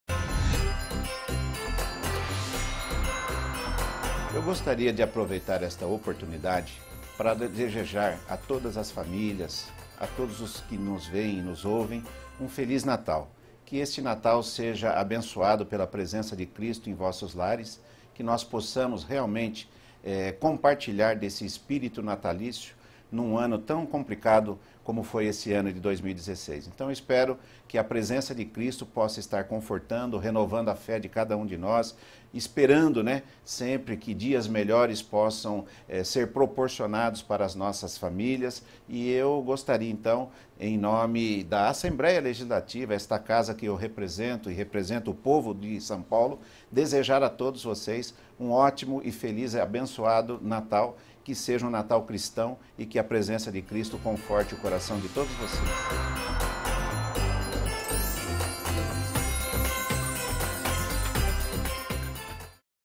Mensagem de Natal do Deputado Marcos Damasio
O deputado Marcos Damasio, do PR , deseja a todos um Feliz Natal.